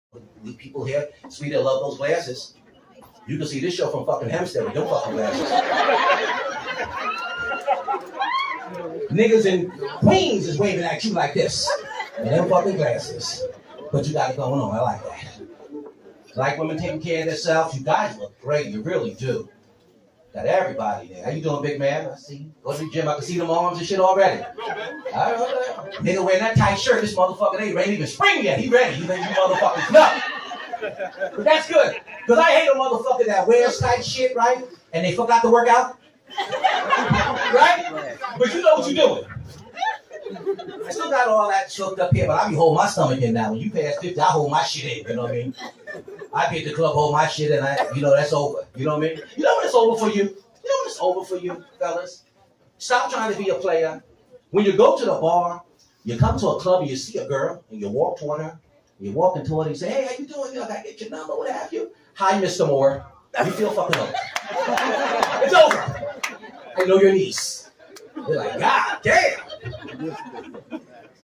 Comedian Howard 'ShowTyme' Moore from a recent Long Island show
See a short clip of Comedian Howard 'ShowTyme' Moore taken from a recent show on Long Island, New York. This was filmed while Howard was headlining at the Governor's Comedy Club.